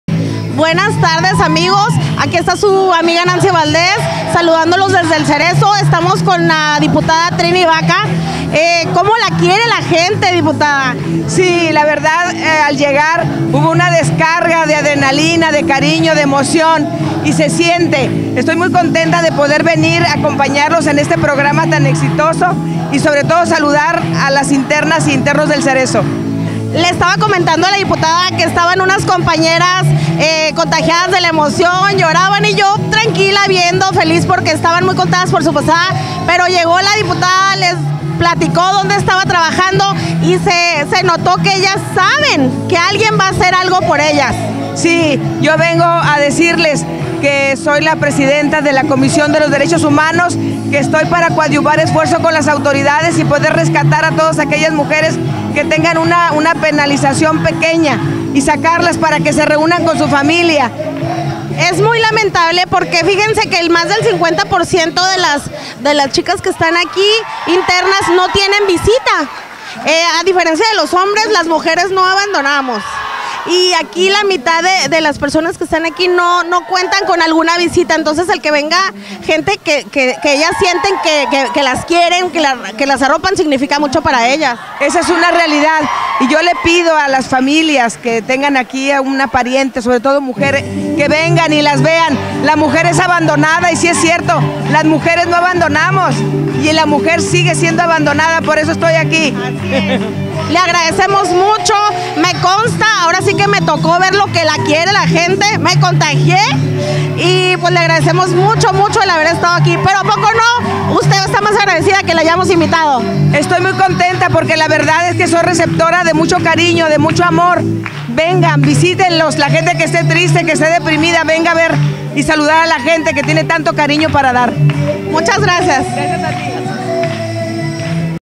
Nancy Valdez entrevista a la Diputada Triny Vaca
Durante la posada realizada para las mujeres en el Cerezo de Mxli. El 15 de Diciembre de 2016